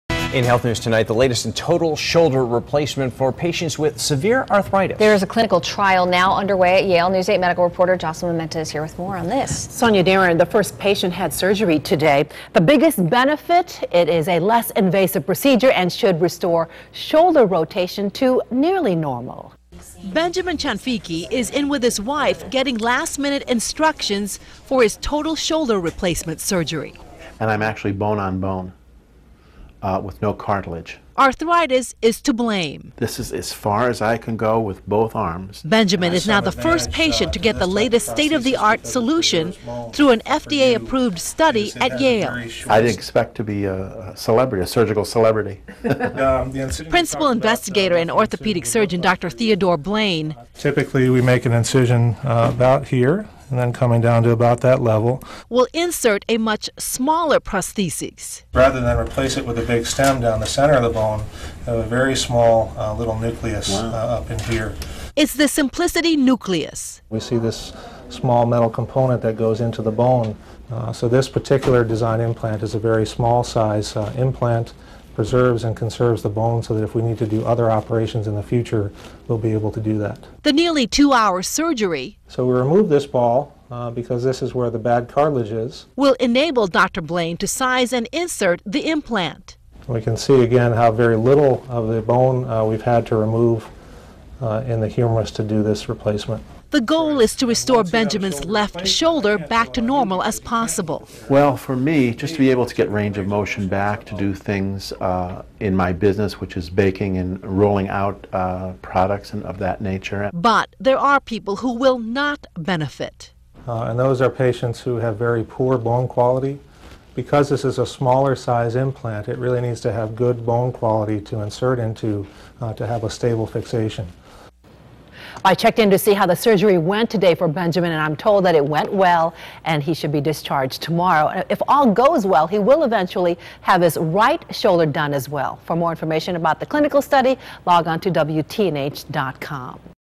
Total shoulder replacement study underway at Yale
The first patient of a Yale study was in surgery Monday and should restore shoulder rotation to nearly normal.